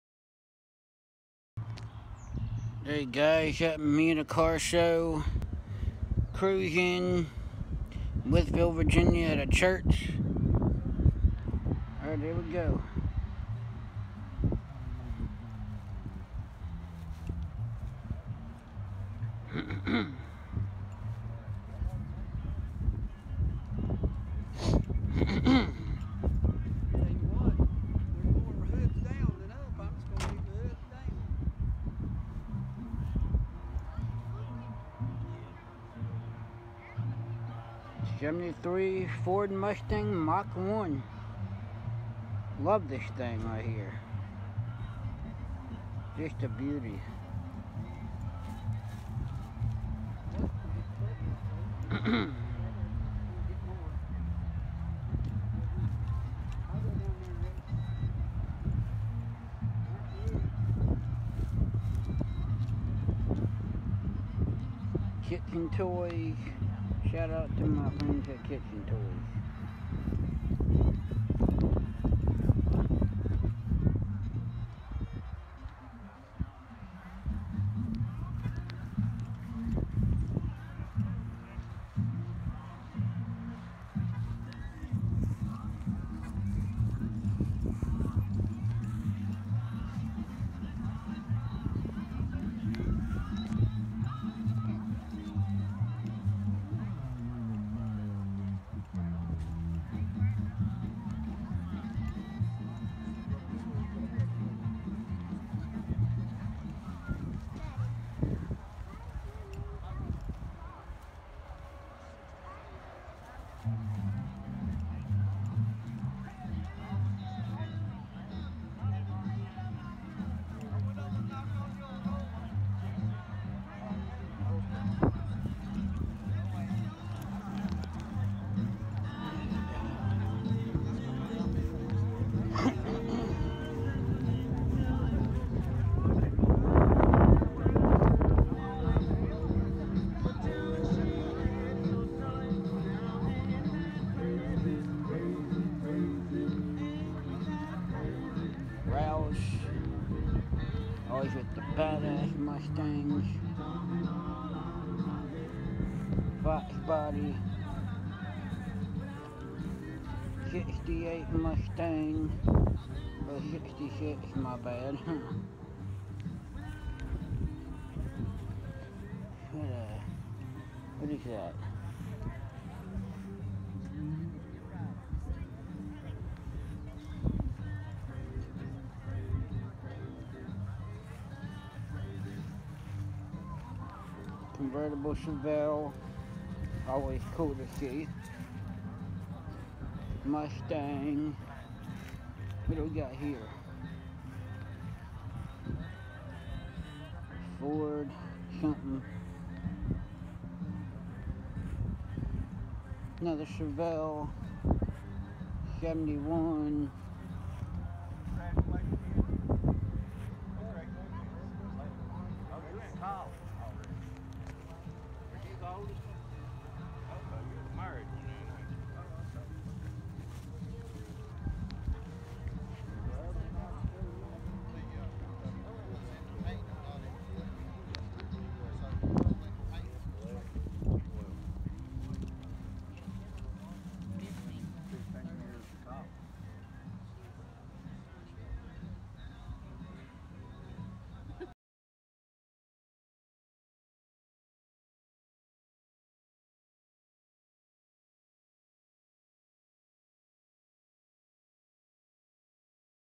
2022 Life Changers Christian Center Car Truck Bike Cruise In
Car show at a church